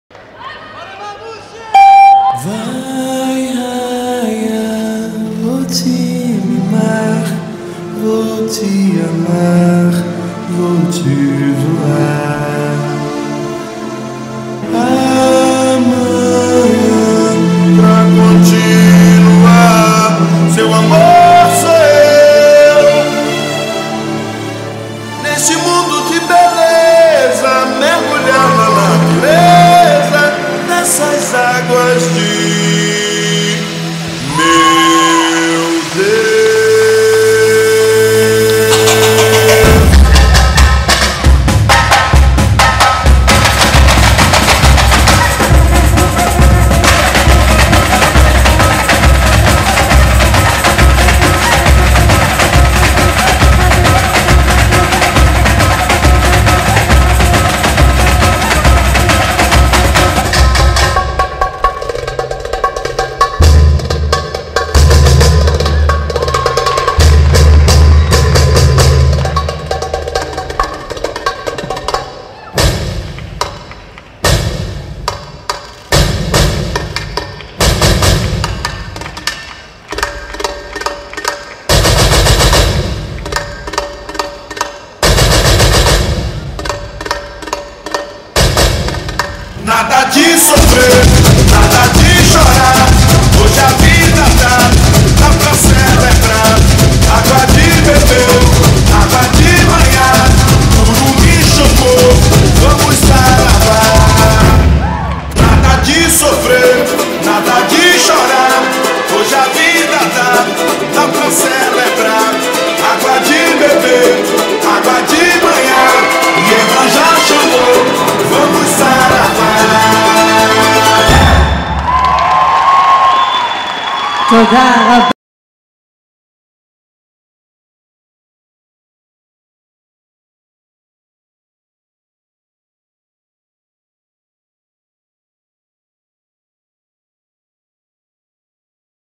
Women's group Portugal - 2019 Acro European silver medallists, dynamic
UEG Official – 29th European Championships in Acrobatic Gymnastics for Seniors & Juniors, 30 October to 3 November 2019, Holon (ISR)

Women's Group: Francisca SAMPAIO MAIA, Francisca MAIA & Barbara SEQUEIRA (POR) 29.070
Dynamic final ... Seniors, 2nd place and silver medallists.